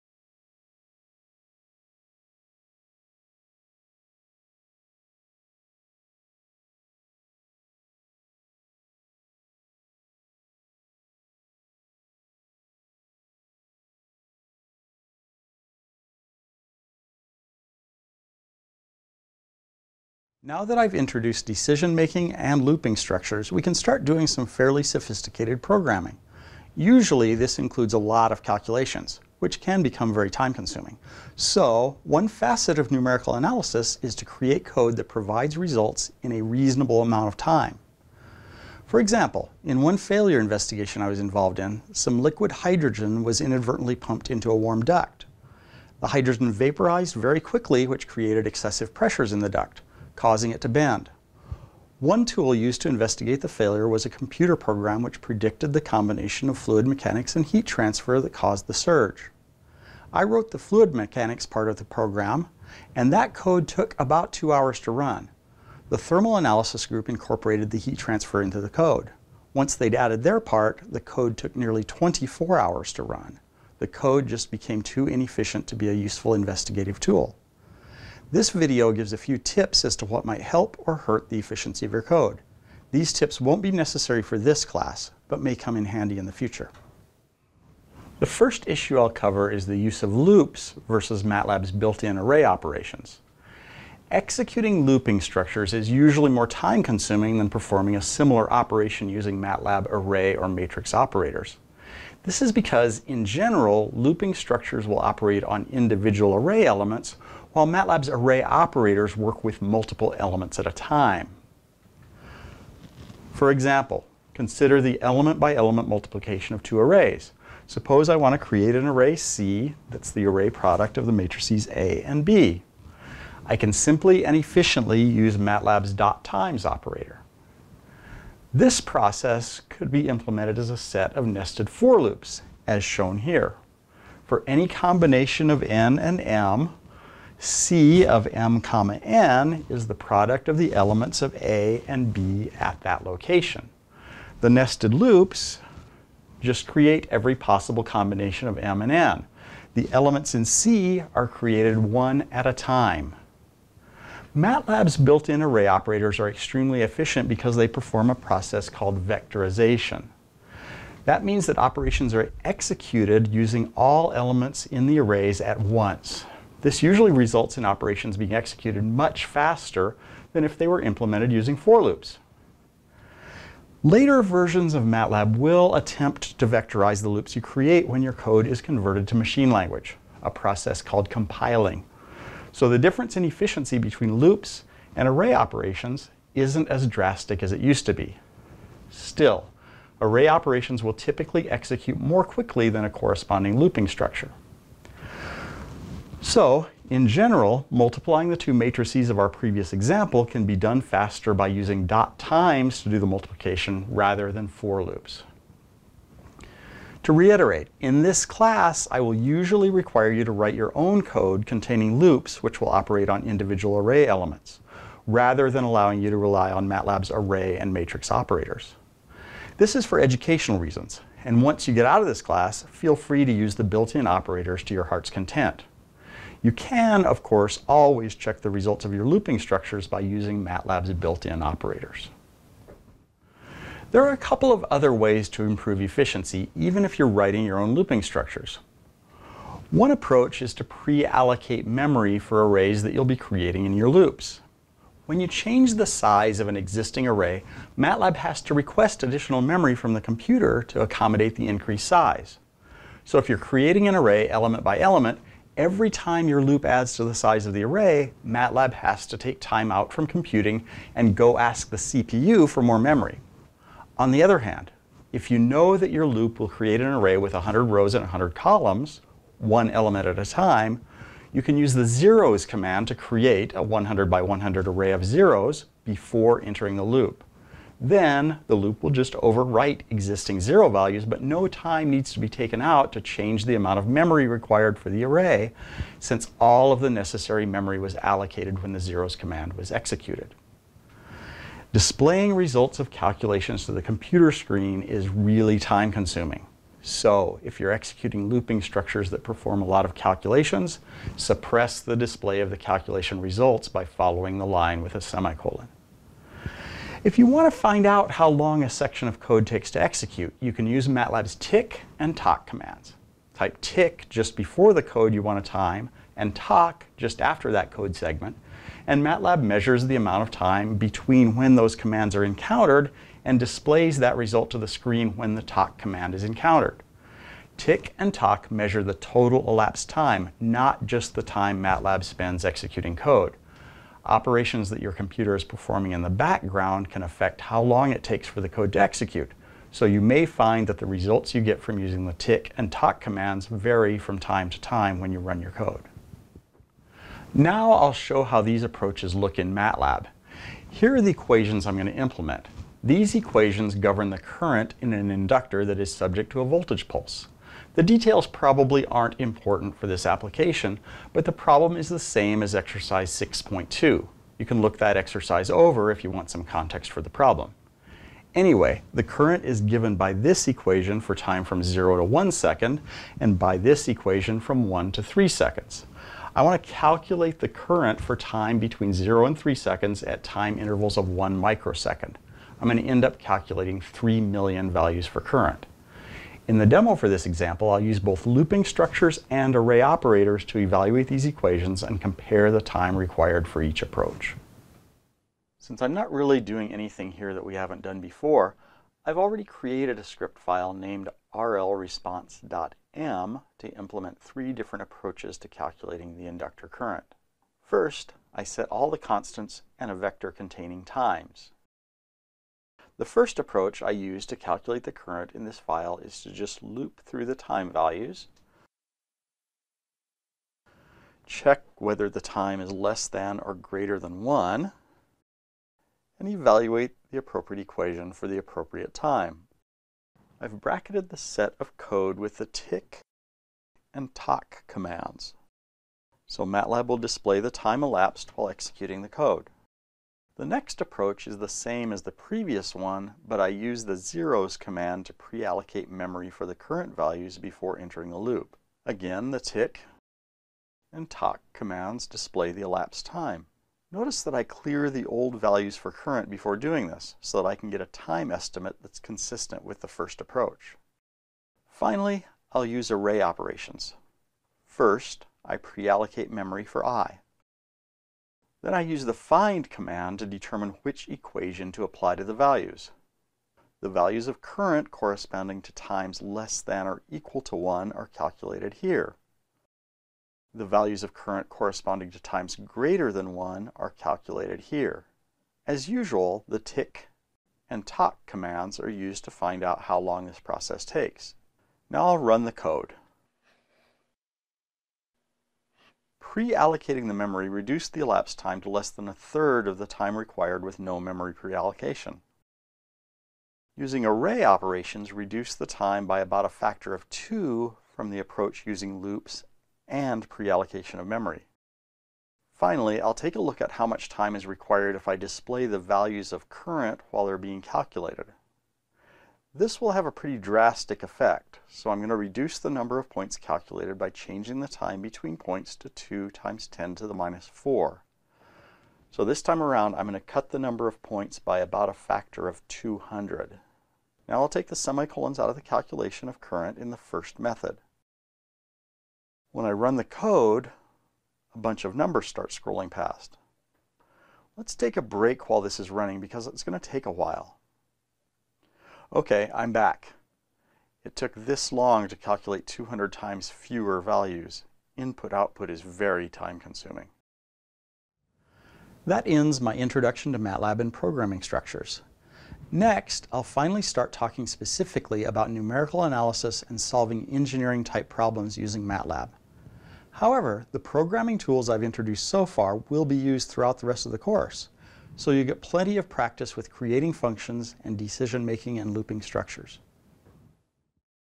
MATLAB Lesson 9.3 - Efficiency
Now that I’ve introduced decision making and looping structures, we can start doing some fairly sophisticated programming.  Usually this includes a LOT of calculations, which can become very time consuming.  So, one facet of numerical analysis is to create code that provides results in a reasonable amount of time.